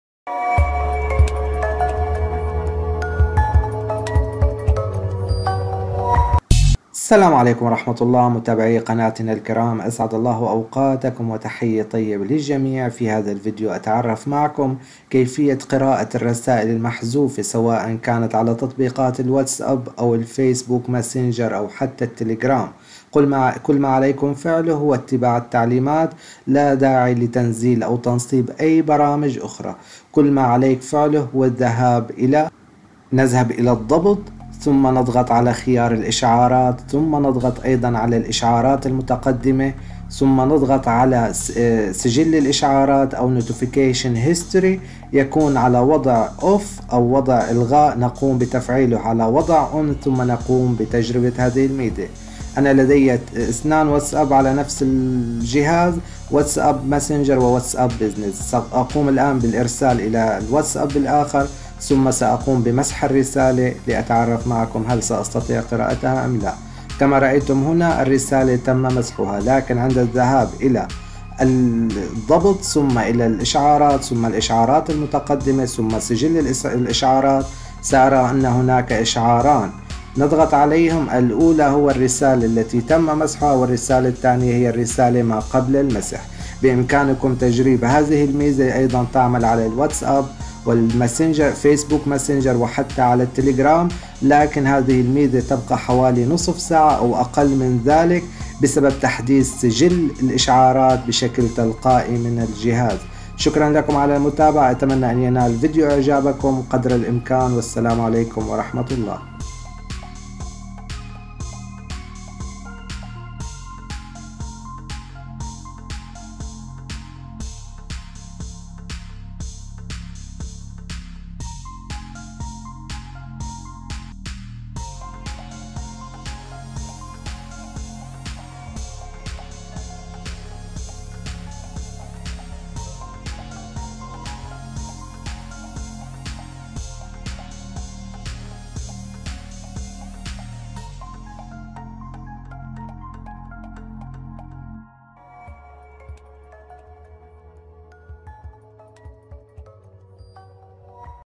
كيف نقرأ الرسائل المحذوفة على الواتساب و التليجرام و الماسنجر و الانستا بعد حذفها بدون برنامج
حيلة بسيطة لقراءة الرسالئل المحدذوفة من دون برنامج بطريقة بسيطة جداً